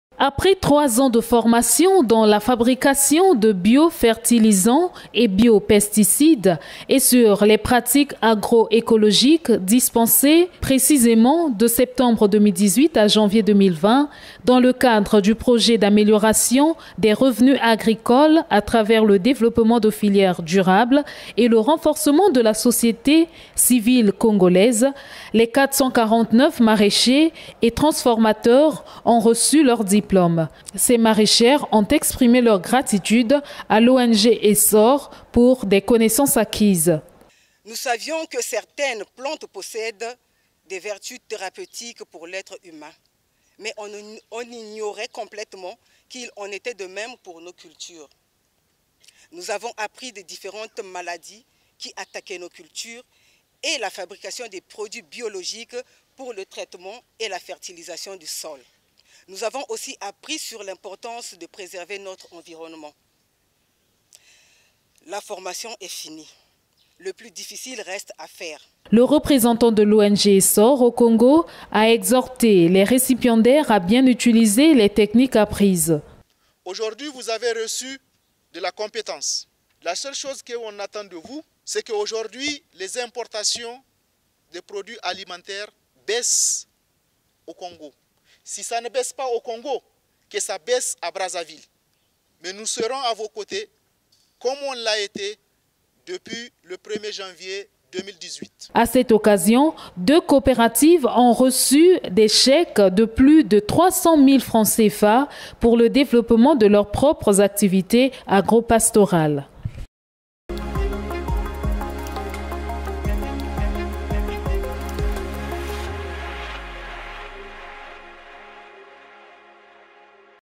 Remise certificats à 449 maraîchers et transformateurs par ESSOR
Après trois (3) ans de formation en fabrication de bio fertilisant, de bio pesticide et sur les pratiques agroécologiques, les participants à cette session de formation organisée dans le cadre du Projet d'amélioration des revenus agricoles à travers le développement des filières durable et le renforcement de la société civile congolaise ont reçu leurs diplômes lors d'une cérémonie tenue à cette occasion. Peu après la réception de leurs diplômes ces participants n'ont pas manqué de mots pour exprimer leur gratitude.